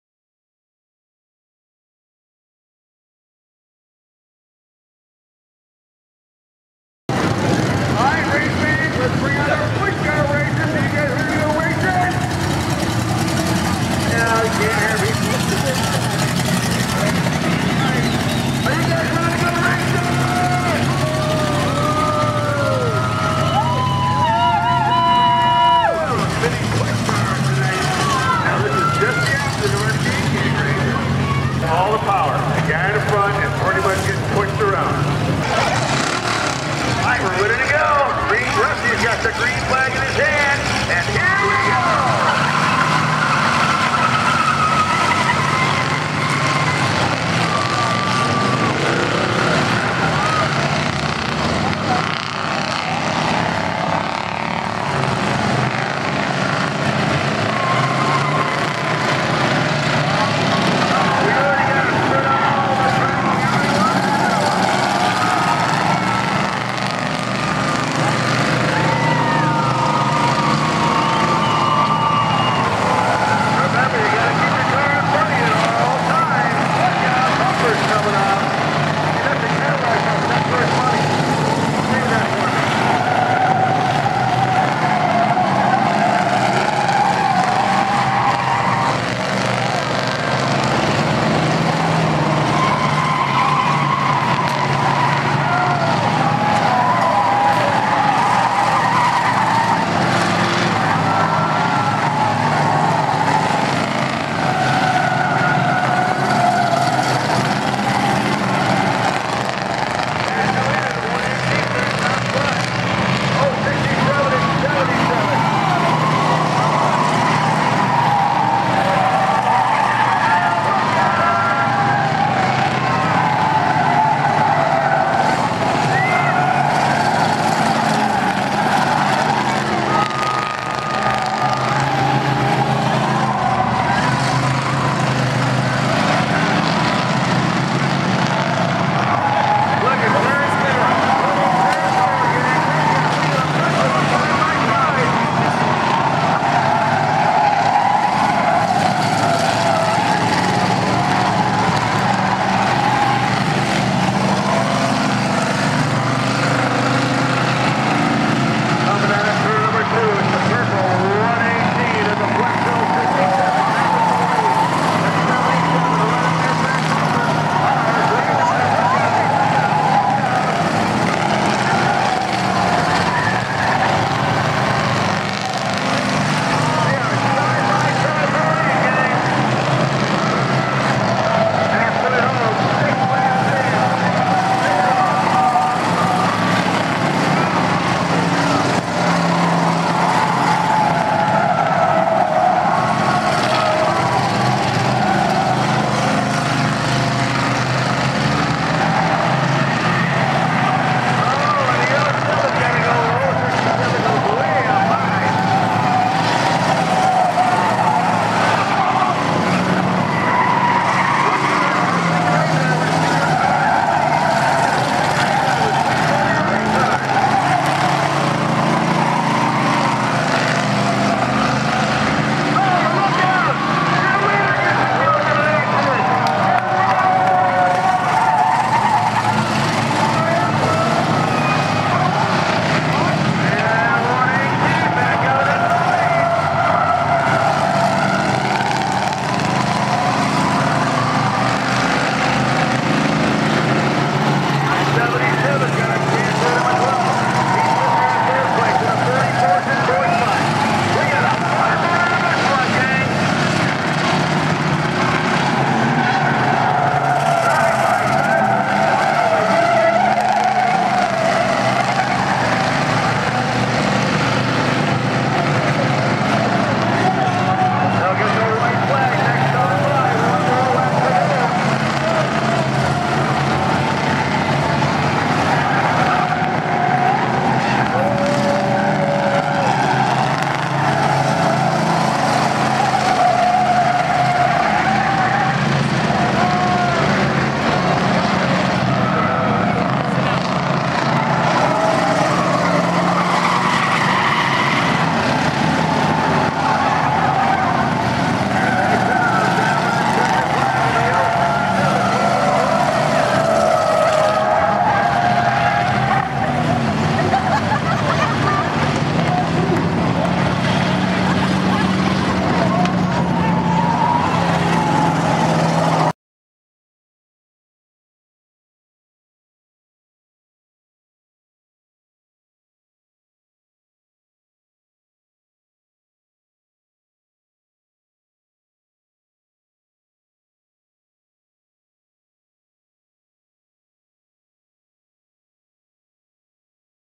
Push Race August 4, 2012 Maximum Destruction Clio Michigan
Push race was ran on August 4, 2012 at the Auto City Event Center in Clio Michigan
Drivers
Cory King #44 Car & Cam King #44 truck
Larry Vanburen #144 car & John King #144 truck